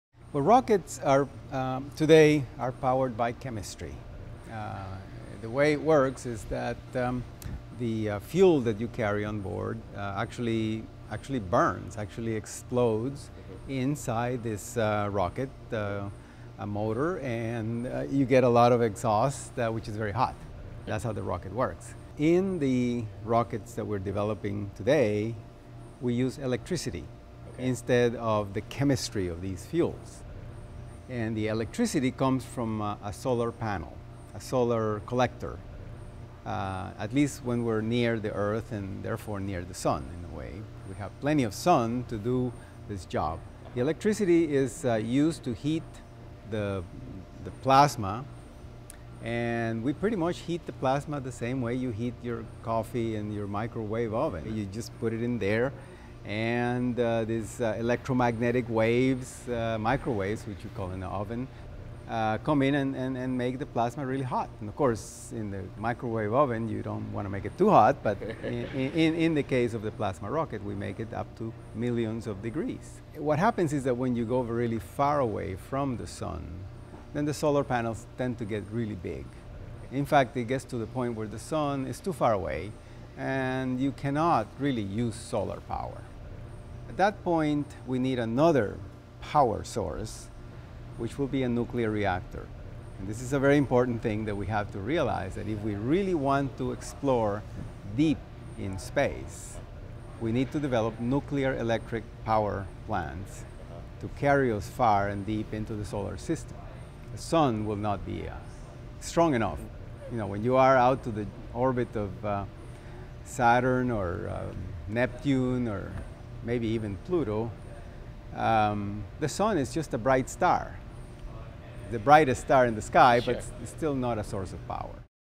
Rockets of the Future
Dr. Franklin Chang-Díaz, president and CEO of Ad Astra Rocket Company, talks about what rockets are and the need for faster rockets for future exploration.